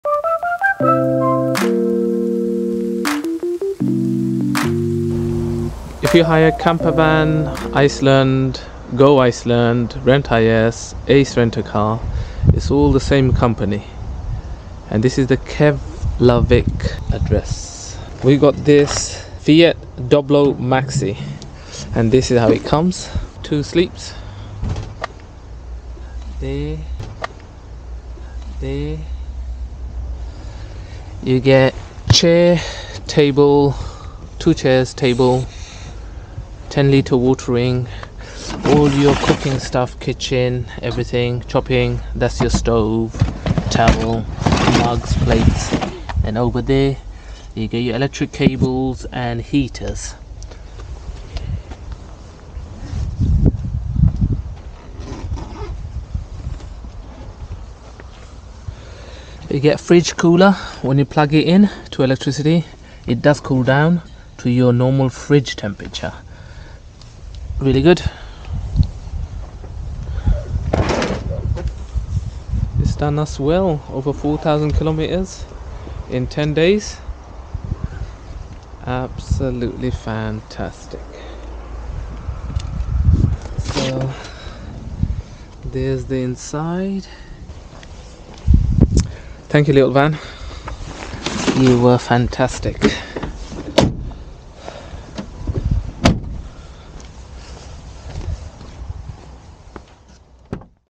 FIAT DOBLO MAXI CAMPER VAN ICELAND ROAD TRIP
This is probably the most popular camper van to hire for 2 and single travelers in Iceland Road Trip. It is very comfortable and lovely vehicle to travel in. Drives like a car. Suitable for many roads. This was perfect for this trip. Next probably go off road and get something suitable for those roads.

Iceland Road Trip. Trip of a life time. Iceland is one of the most beautiful places on Earth. Been wanting to visit and do this for many years, FINALLY here so let's go and explore.
Travel video of the main things saw and places visited. This was a 10 day road trip. Wanted to mainly do the Road 1 Ring Road route going counter clockwise. The camper van was not suitable for off road so had to stay on the main acceptable roads.

Iceland is a Nordic island country in the North Atlantic Ocean and in the Arctic Ocean. Iceland is the most sparsely populated country in Europe. Iceland's capital and largest city is Reykjavík, which (along with its surrounding areas) is home to over 65% of the population. Iceland is the biggest part of the Mid-Atlantic Ridge that rises above sea level, and its central volcanic plateau is erupting almost constantly. The interior consists of a plateau characterised by sand and lava fields, mountains, and glaciers, and many glacial rivers flow to the sea through the lowlands. Iceland is warmed by the Gulf Stream and has a temperate climate, despite a high latitude just outside the Arctic Circle. Its high latitude and marine influence keep summers chilly, and most of its islands have a polar climate.

There is a lot of footage and videos to edit and be posted here. Looking very forward to sharing all this with you.